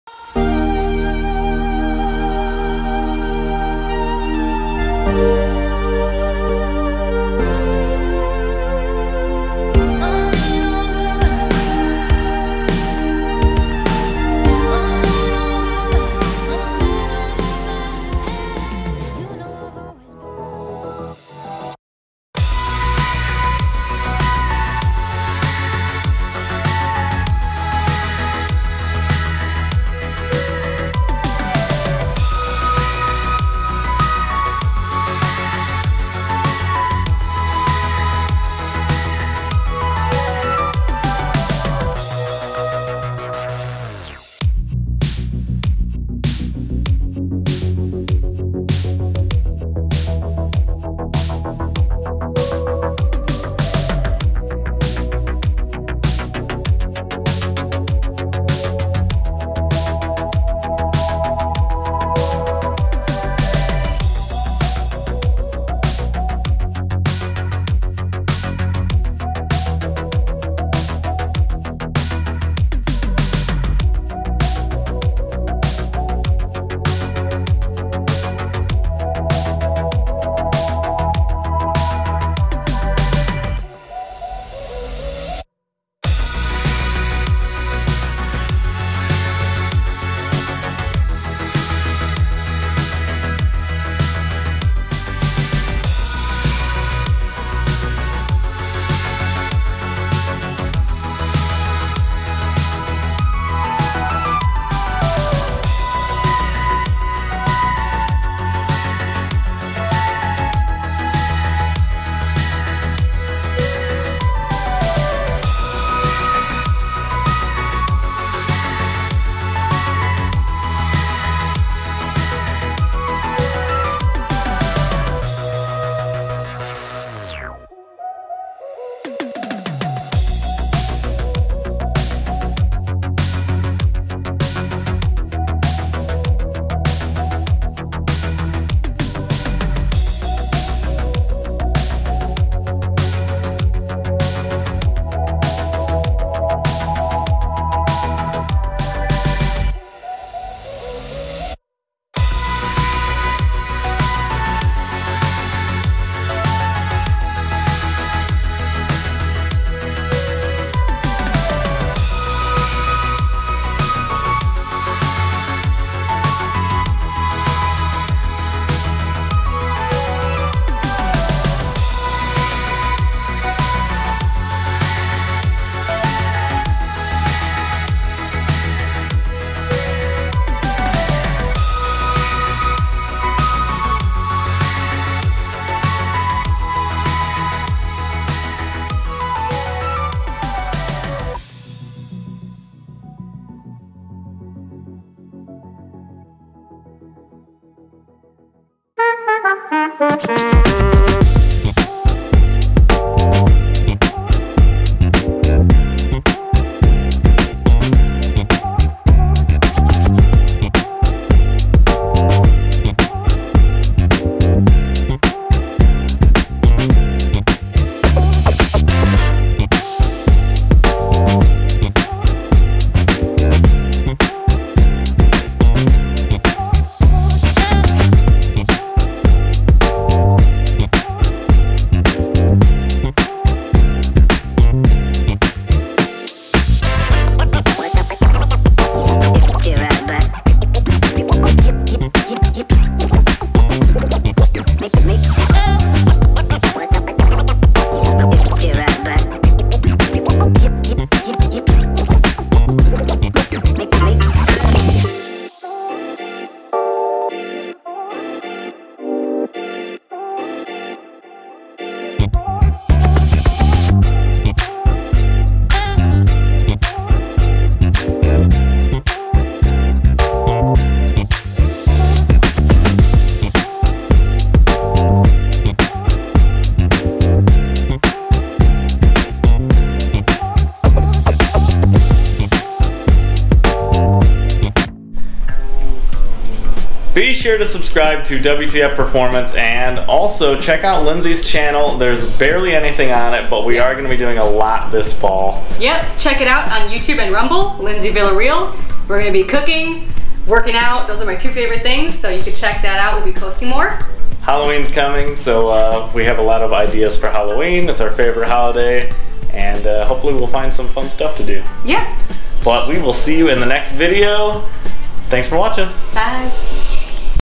Bathroom and Laundry room makeover Before and After!
If you don't want to watch all three parts of our Bathroom and Laundry room remodel, then enjoy 5 minutes of before and after shots of this budget makeover!

Joel: wtfperformance
Lindsay: lindsayvillarreal9